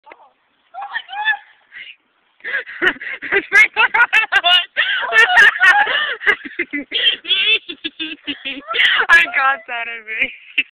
sprinklers turn on, on kelly.
i know its bad quality, i video taped with my old phone !
this is from june 2009